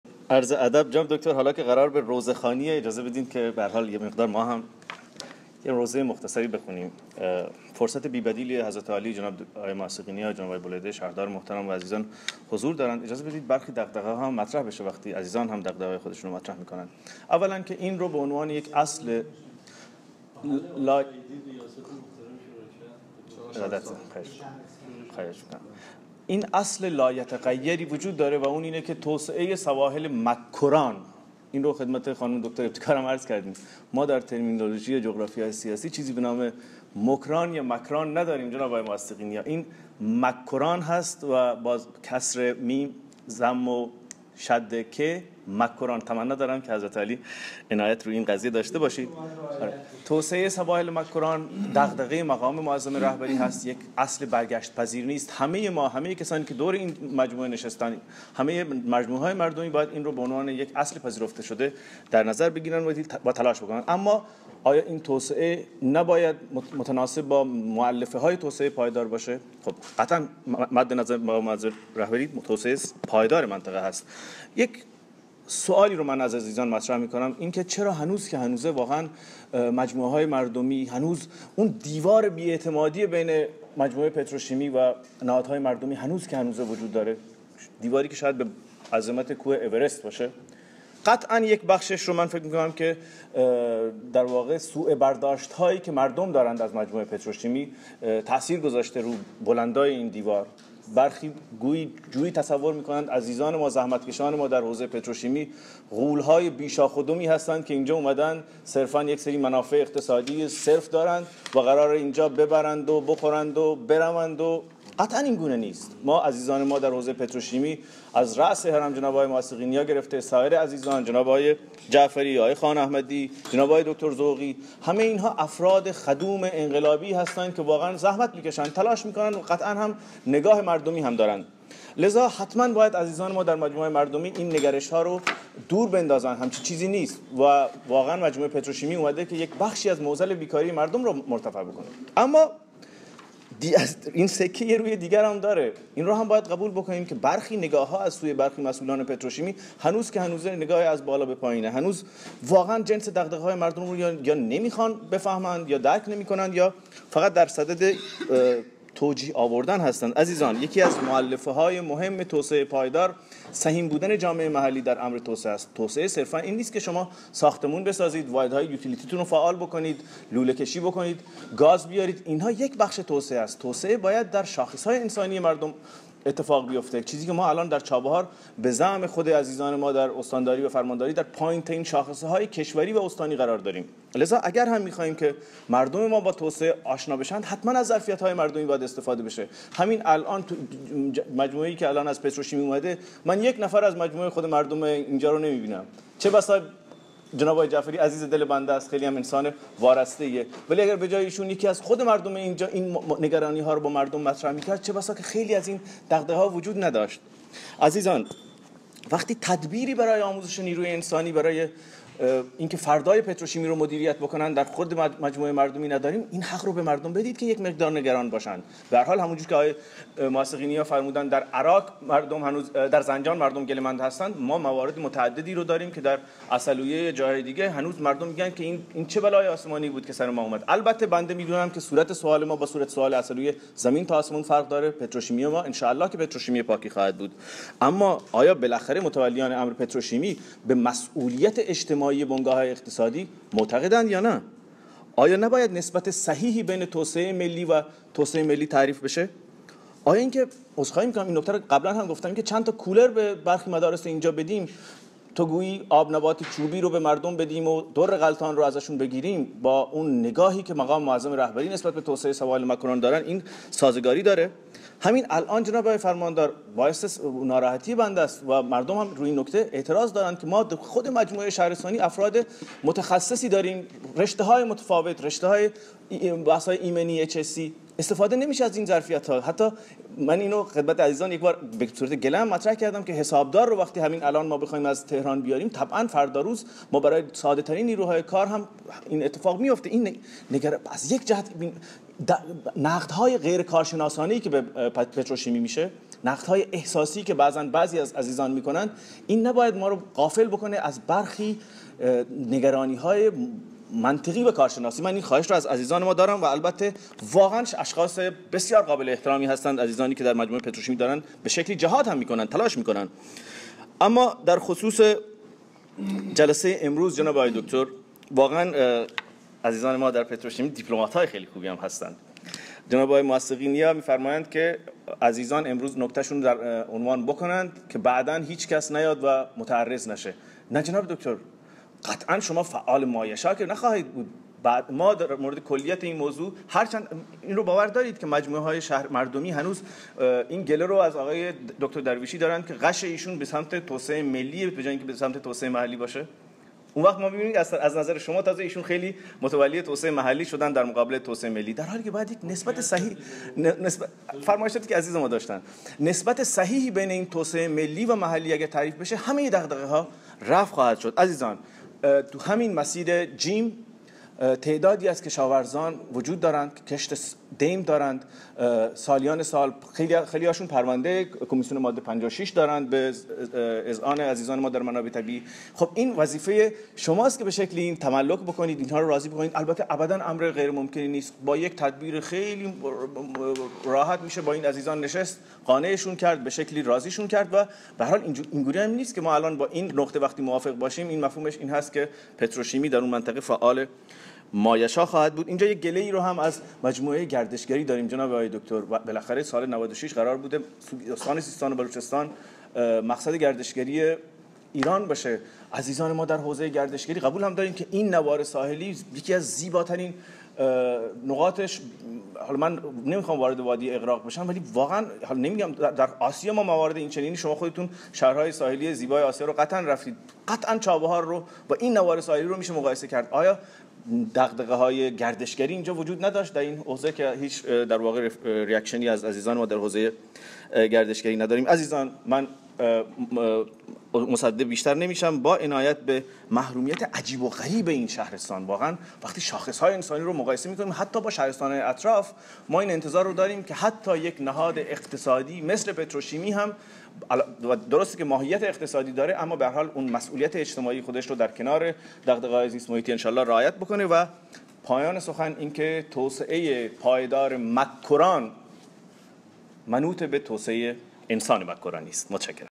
سخنان معین الدین سعیدی، در مورد طرح توسعه سواحل مکران
ویدیو: سخنان معین الدین سعیدی (عضو شورای شهر چابهار) در مورد طرح توسعه سواحل مکران
کاری از: شبکه جهانی توحید